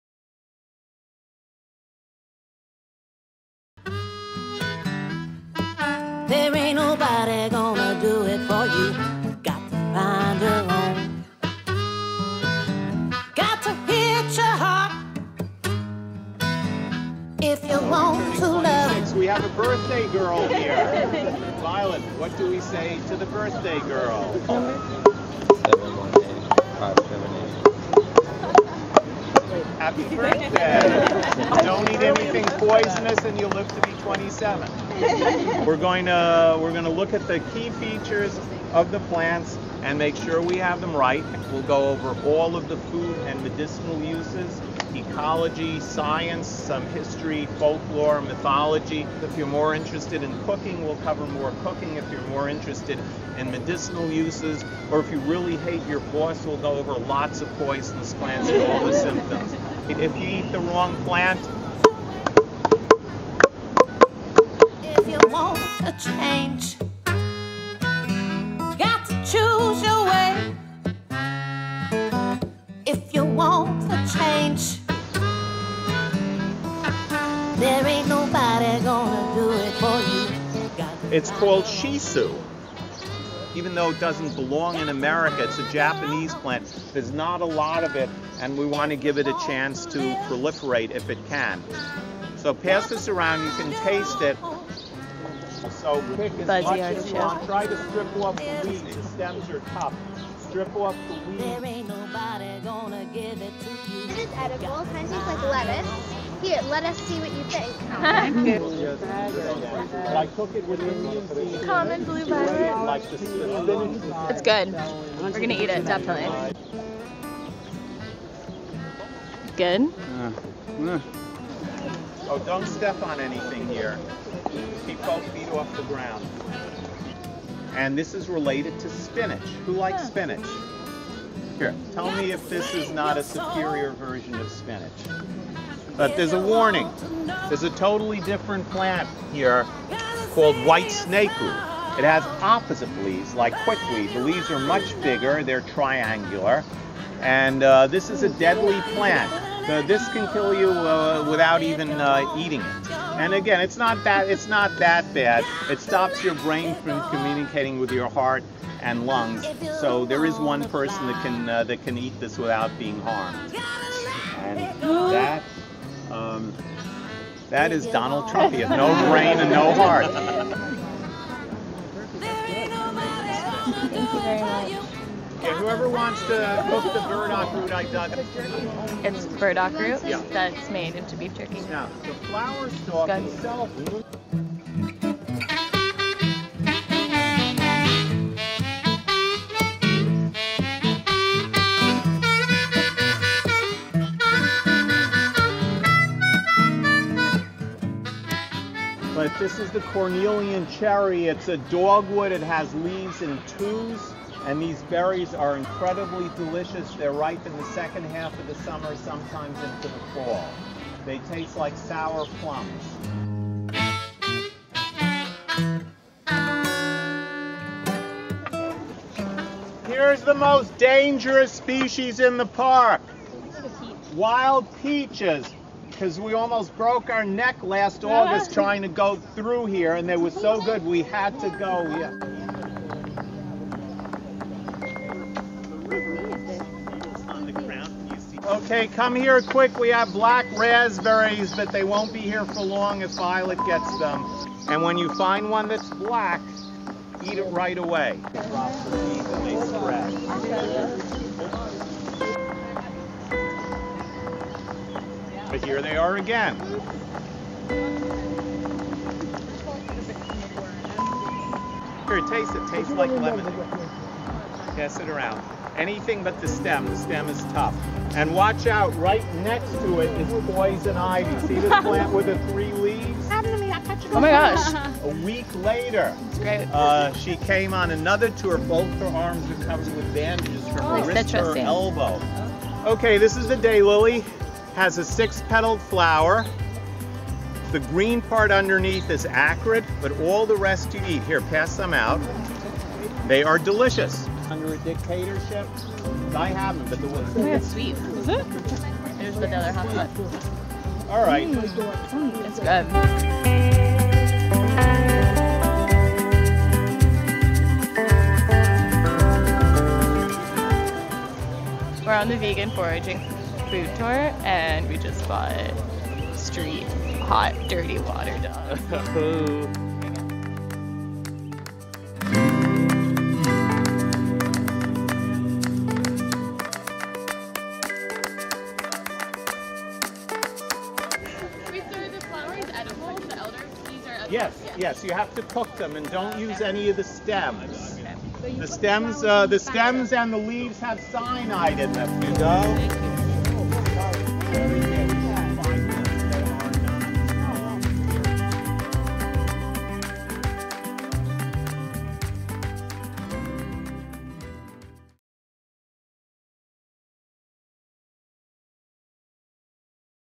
PlantPop NYC: "Wildman" Steve Brill
In the latest episode of PlantPop NYC, Stephen and his assistant, Sam, went on a foraging tour led by "Wildman" Steve Brill. Steve's extensive knowledge of edible and medicinal plants with a side of humor makes for an educational and fun tour.

This episode was filmed by Stephen Farris.